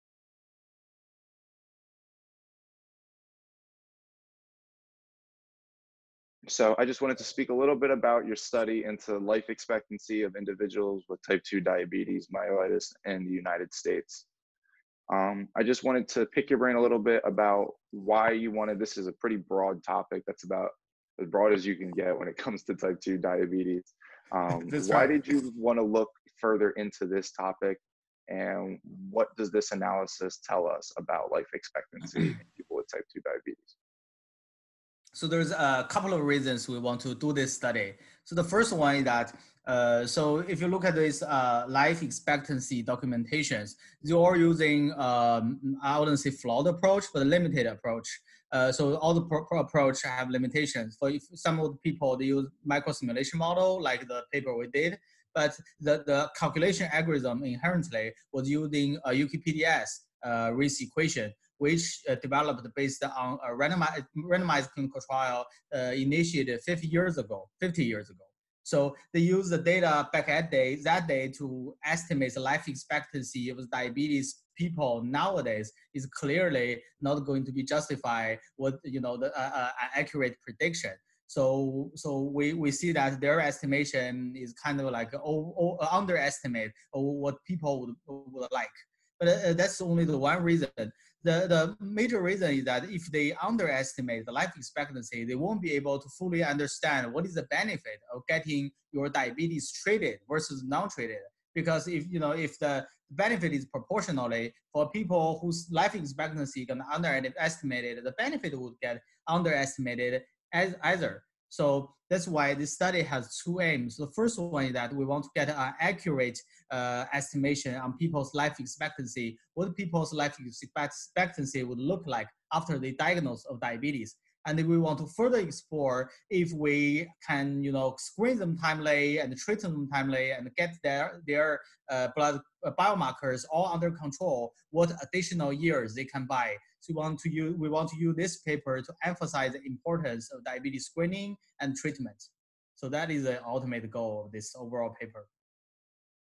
Hui Shao, MD, PhD: Life Expectancy in Patients with Type 2 Diabetes Mellitus
Discussing a look at the current life expectancy of patients with T2DM with an assistant professor from the University of Florida’s College of Pharmacy.